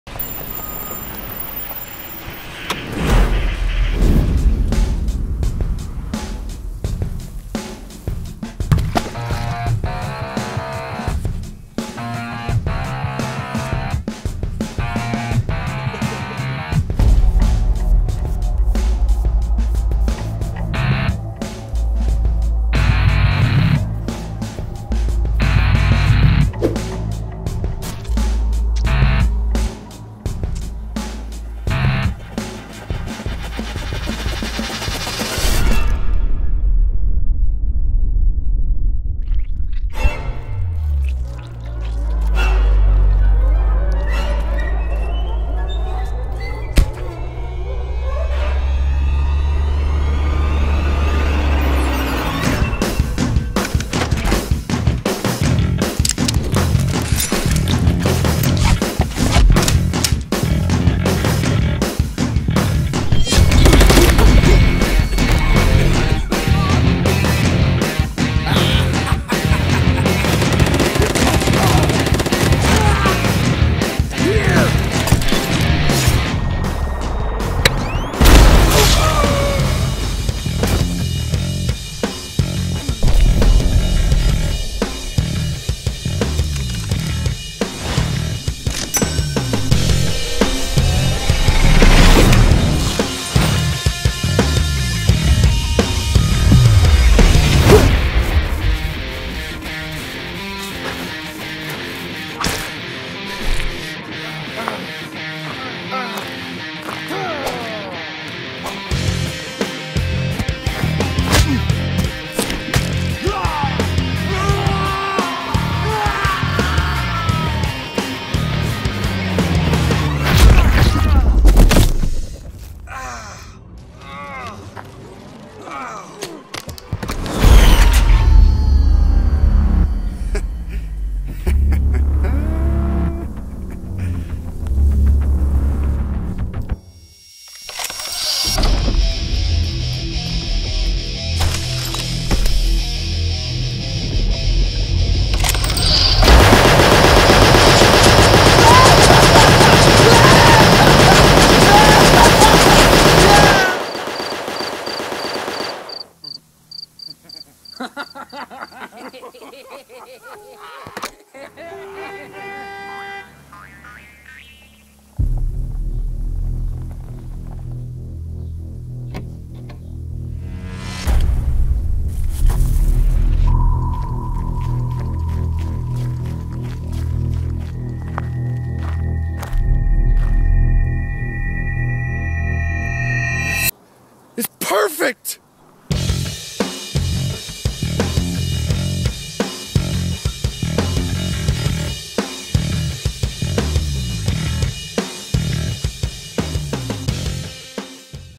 Chroma Crisis - A Splatoon Fight
Painters fight for their turf, splatoon style!

Painters: Seth McMurry,

Chase Rackley,

Michael Mayo

Fabulous Property Owner: Andrew McMurry